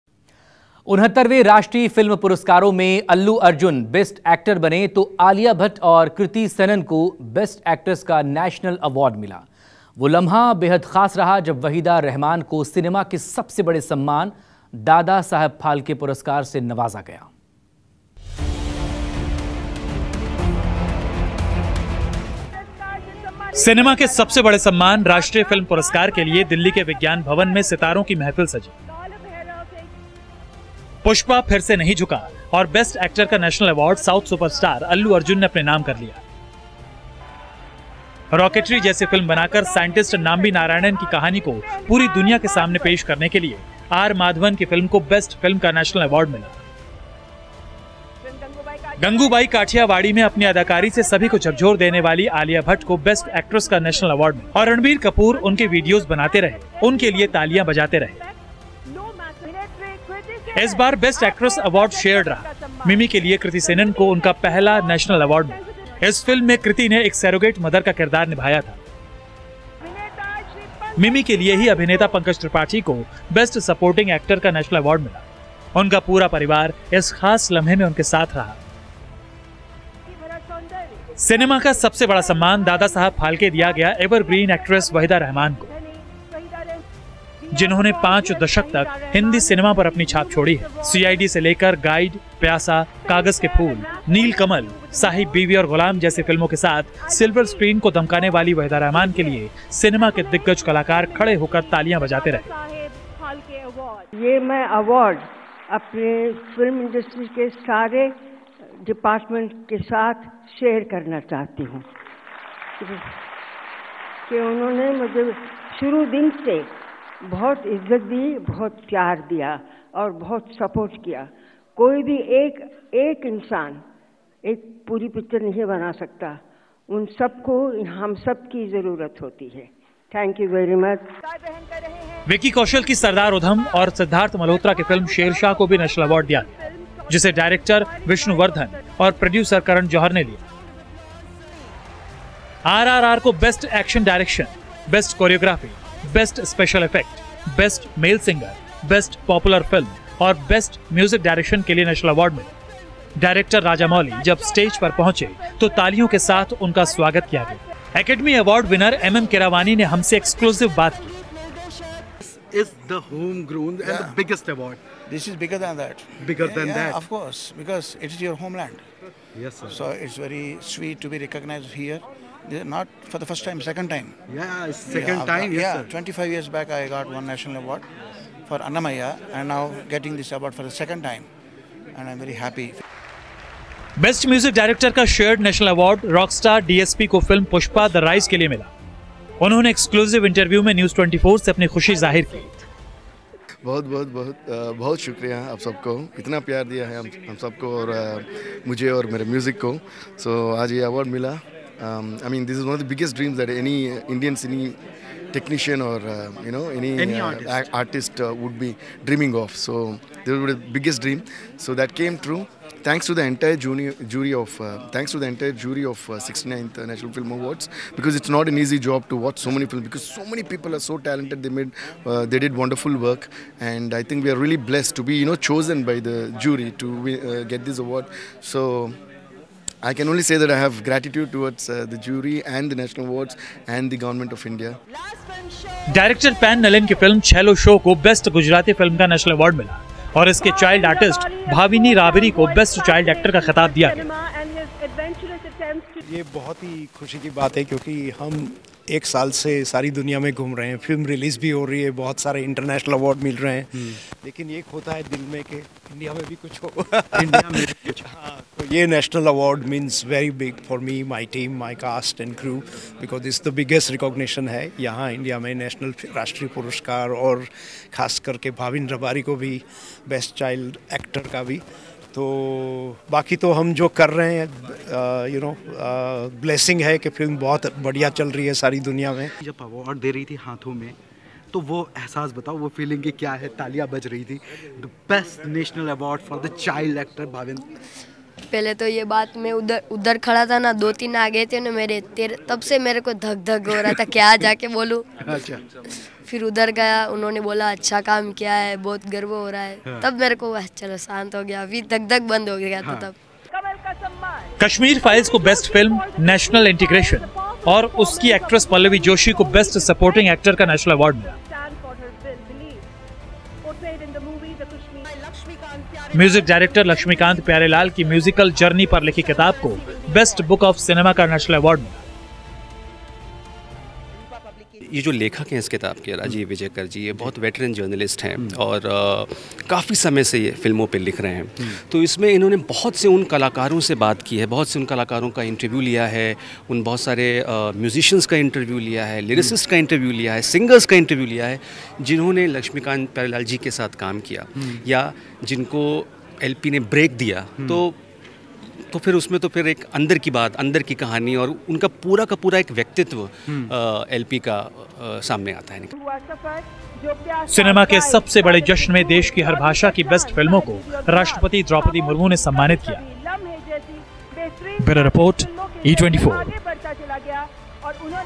National Film Awards में बजा RRR का डंका… Allu Arjun, Alia Bhatt, Pankaj Tripathi को नेशनल अवॉर्ड
69th National Film Awards में बजा फिल्म RRR का डंका… Allu Arjun, Alia Bhatt, Pankaj Tripathi को मिला नेशनल अवॉर्ड